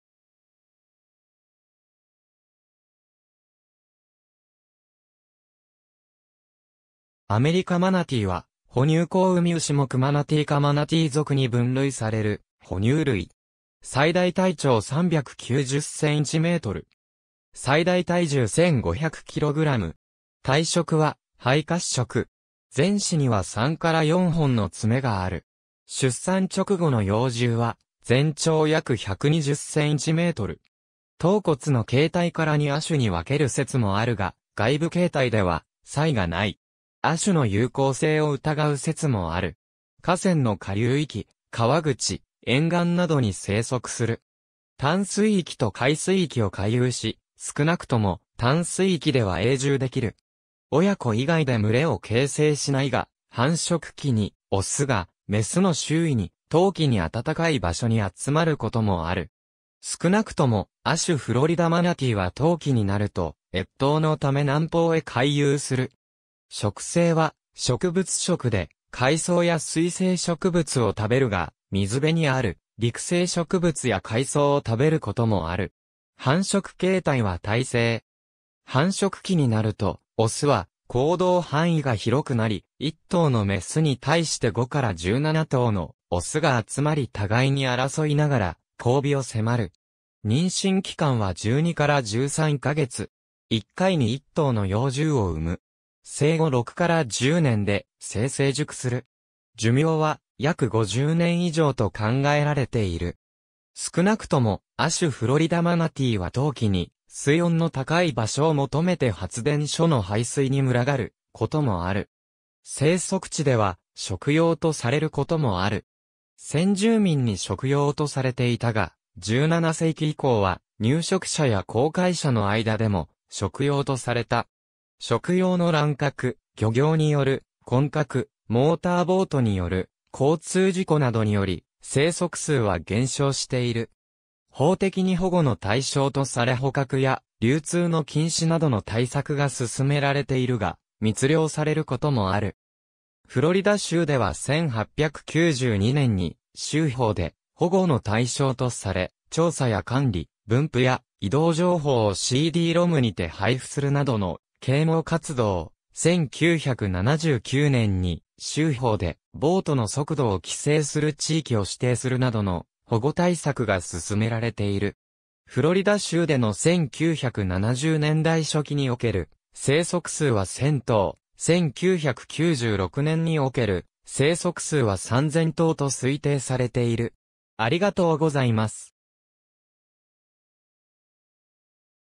アメリカマナティー
アメリカマナティー（Trichechus manatus）は、哺乳綱海牛目マナティー科マナティー属に分類される哺乳類。
最大体長390センチメートル。
最大体重1,500キログラム。
体色は灰褐色。
前肢には3-4本の爪がある。
出産直後の幼獣は全長約120センチメートル。
頭骨の形態から2亜種に分ける説もあるが、外部形態では差異が無い。
亜種の有効性を疑う説もある。
河川の下流域、河口、沿岸などに生息する。
淡水域と海水域を回遊し、少なくとも淡水域では永住できる。
親子以外で群れを形成しないが、繁殖期にオスがメスの周囲に、冬季に温かい場所に集まる事もある。
少なくとも亜種フロリダマナティーは冬季になると越冬のため南方へ回遊する。
食性は植物食で、海草（アマモなど）や水生植物（ホテイアオイなど）を食べるが、水辺にある陸生植物や海藻を食べる事もある。
繁殖期になるとオスは行動範囲が広くなり、1頭のメスに対して5-17頭のオスが集まり互いに争いながら交尾を迫る。
妊娠期間は12-13か月。
1回に1頭の幼獣を産む。
生後6-10年で性成熟する。
寿命は約50年以上と考えられている。
少なくとも亜種フロリダマナティーは冬季に水温の高い場所を求めて発電所の排水に群がることもある。
生息地では食用とされる事もある。
先住民に食用とされていたが、17世紀以降は入植者や航海者の間でも食用とされた。
食用の乱獲、漁業による混獲、モーターボートによる交通事故などにより生息数は減少している。
法的に保護の対象とされ捕獲や流通の禁止などの対策が進められているが、密猟される事もある。
フロリダ州では1892年に州法で保護の対象とされ、調査や管理、分布や移動情報をCD-ROMにて配布するなどの啓蒙活動、1979年に州法でボートの速度を規制する地域を指定するなどの保護対策が進められている。
フロリダ州での1970年代初期 における生息数は1,000頭、1996年における生息数は3,000頭と推定されている。